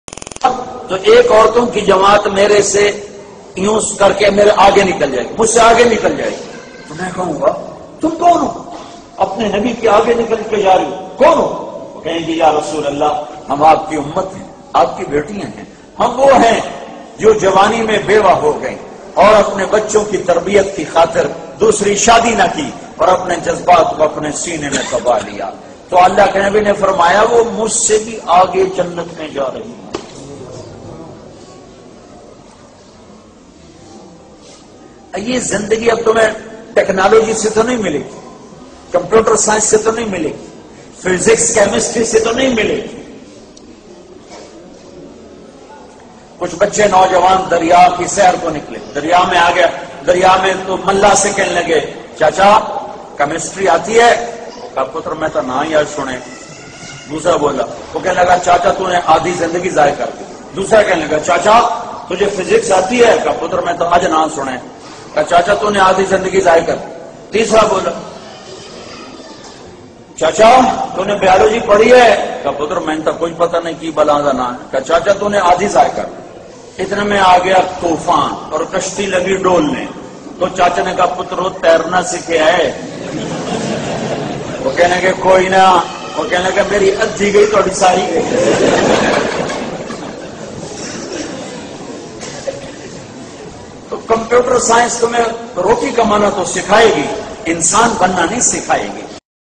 بیوہ کا مرتبہ
آپ بھی بیوہ کا خیال رکین
کسی بیوہ کو کبھی بھی د کھ مت پھچاٰ ئین
رب پاک کے پاس پاکدامن بیوہ کا بڑا مرتبہ  ھے